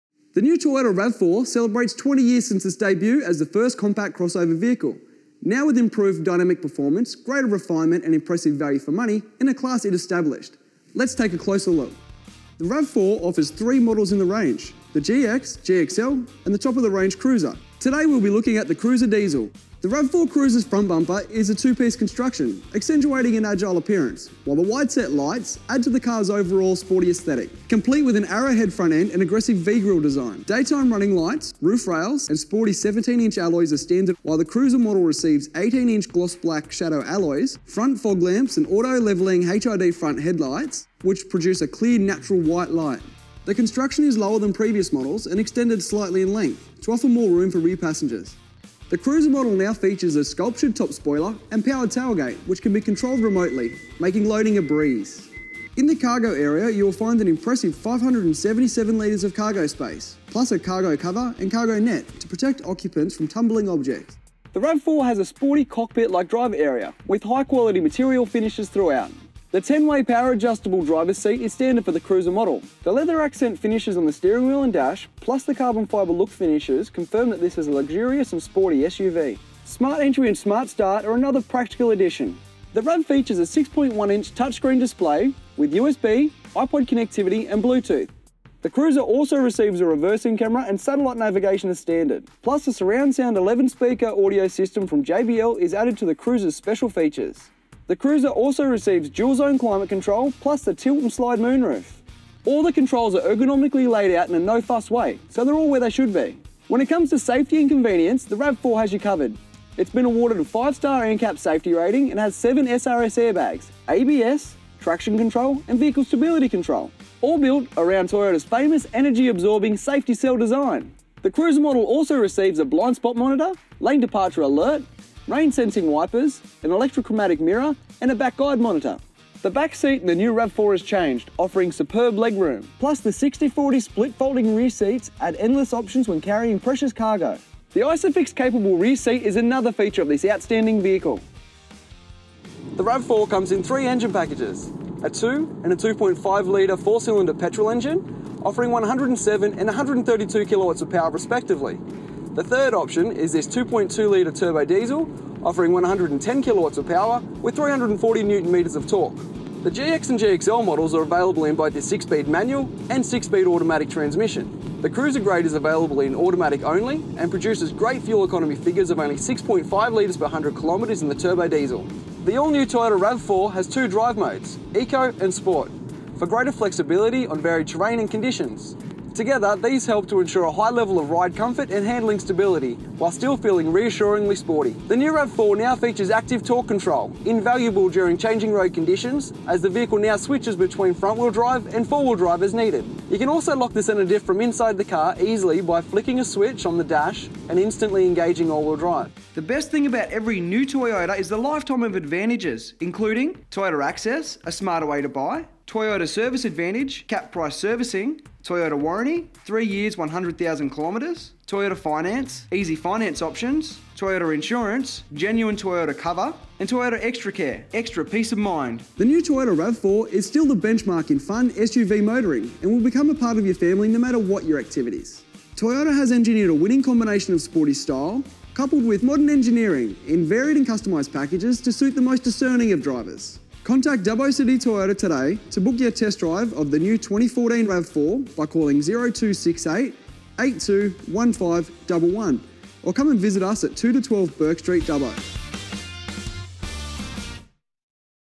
CAR REVIEW- 2014 TOYOTA RAV4 - DUBBO CITY TOYOTA [AUSTRALIA] [HD 1080]
The new Toyota RAV4 celebrates 20 years since it's debut as the first compact cross over vehicle.

DUBBO CITY TOYOTA
2-12 Bourke St, Dubbo, NSW 2830

LET'S GO DUBBO!

Created, Produced & Managed by Cartelux Pty Ltd.